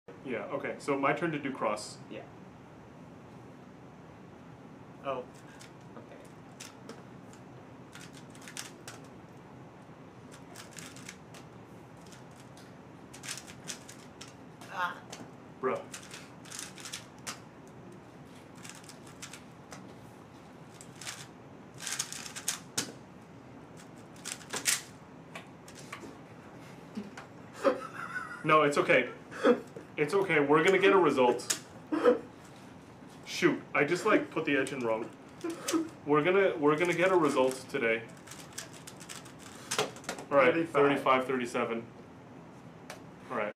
beautiful factory solve with phil yu
This was our second attempt and the first was a DNF. Our best solve was a 13, which you can watch

Cube: WRM 2019 with "The Thing"

Be sure to check out TheCubicle on Twitch for more content like this in the future!